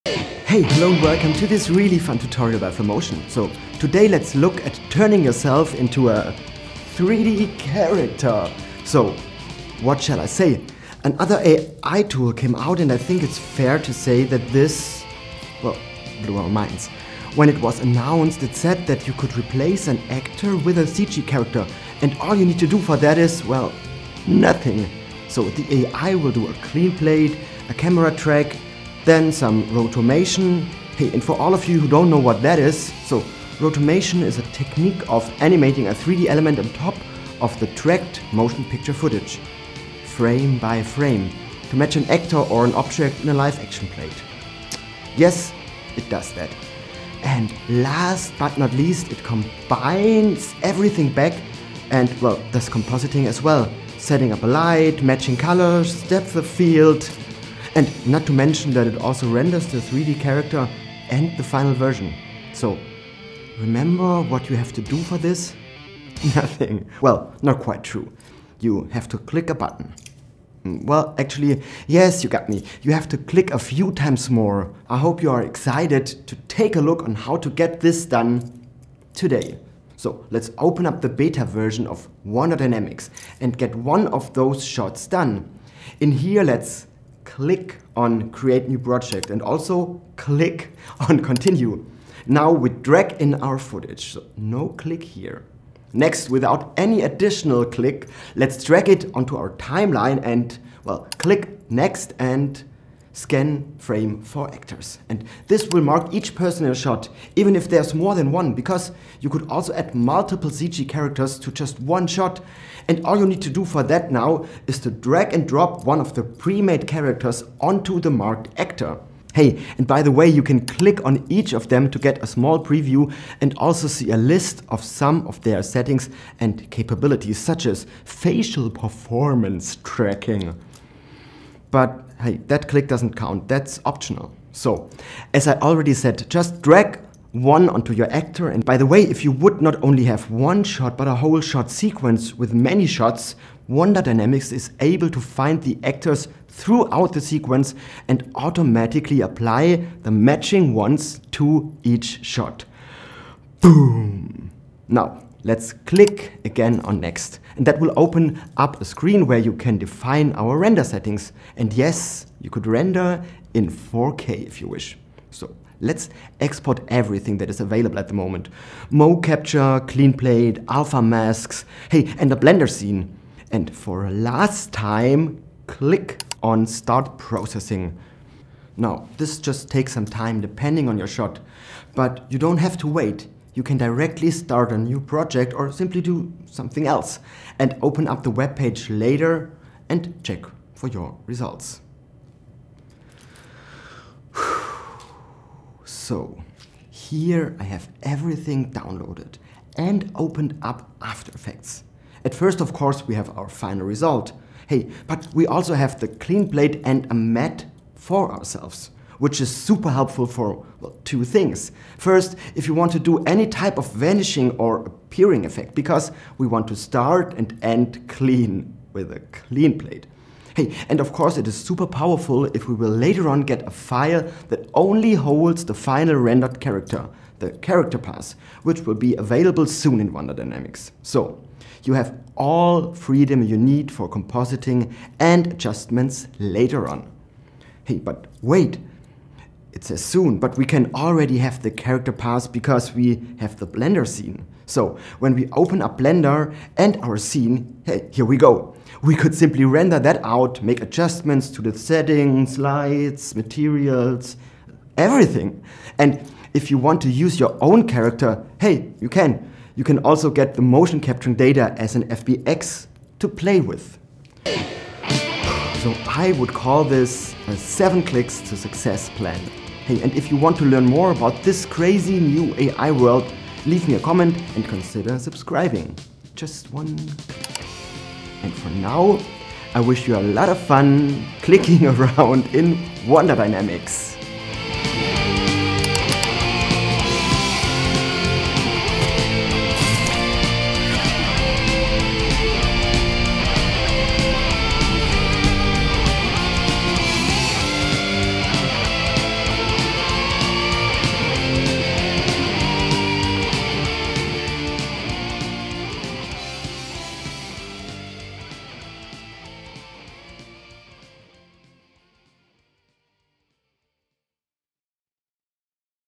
Wonder Dynamics Tutorial - MOTION CAPTURING and CG CHARACTERS made easy
Wonder Dynamics Tutorial - Motion Capturing Motion Capture made easy. AI CG Character animation.

In this new tutorial by flomotion I am going to show you how you can use "Wonder Dynamics" to motion capture, animate, and composite CG characters.
Wonder Dynamics is part of Wonder Studio.
You will get an alpha matte, a clean plate, final result and MoCap Motion Capturing Data as FBX file as well as the blender scene file.

You will find explosions as well as many different vfx elements here.

No more tracking suits / MoCap Suits anymore.

What is wonder dynamics?
An AI tool that automatically animates, lights and composes CG characters into a live-action scene.
The system automatically detects the actor’s performance based on single-camera footage. Then, it takes that performance and transfers it to the CG character of your choice — automatically animated, lit, and composed.